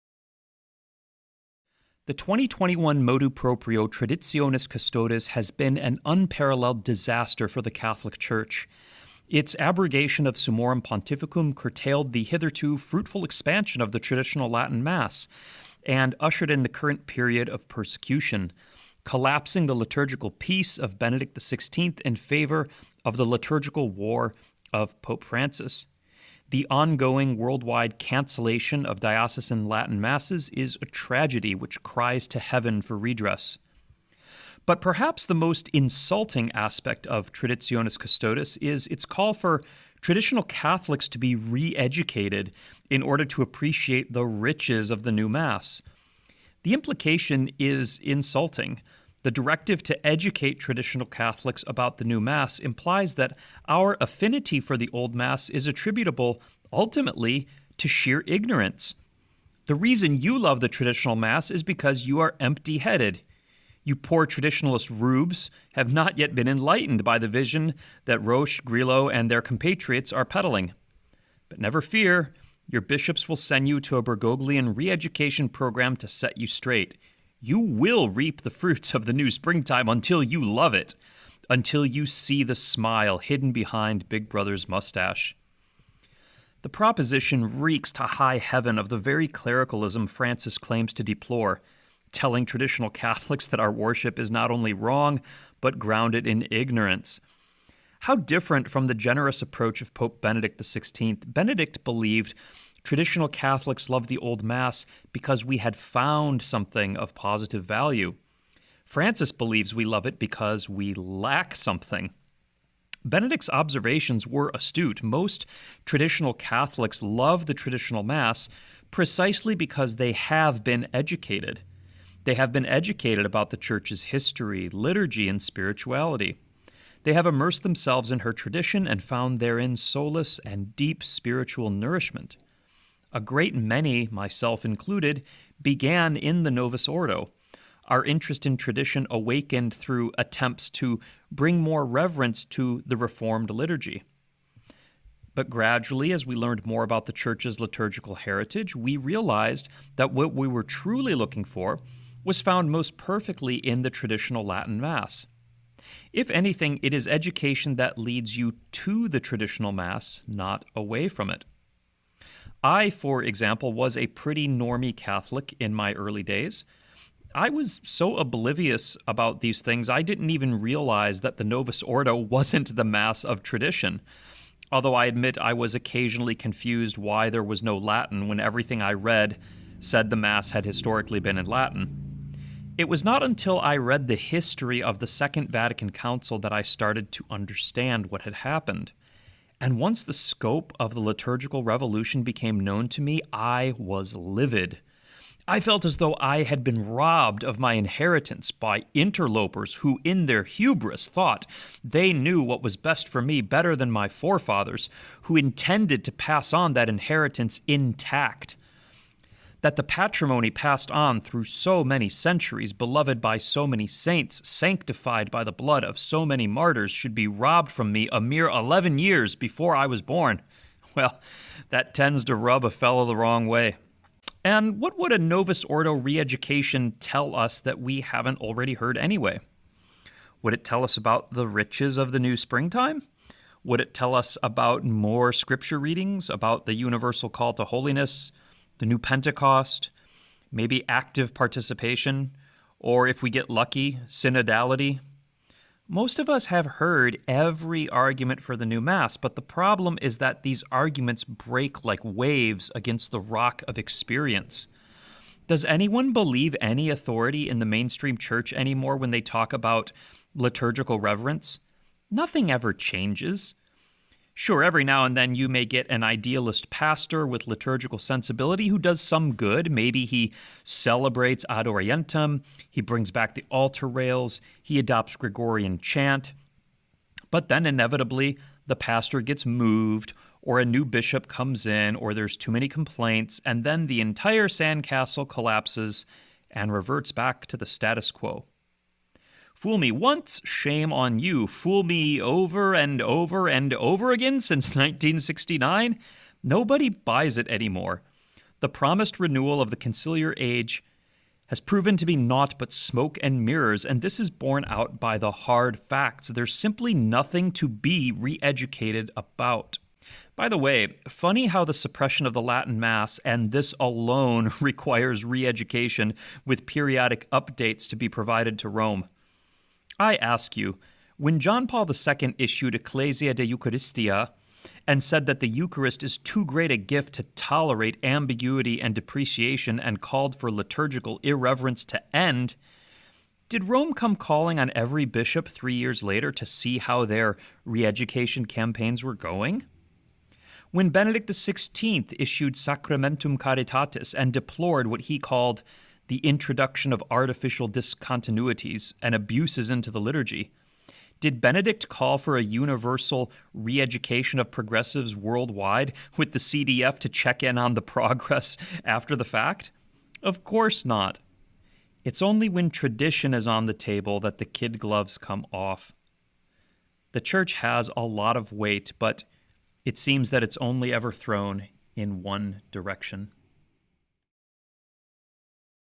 The Insulting Clericalism of Traditionis Custodes
The insulting implication of Traditionis Custodes that Catholics who like the old Mass do so because they are stupid.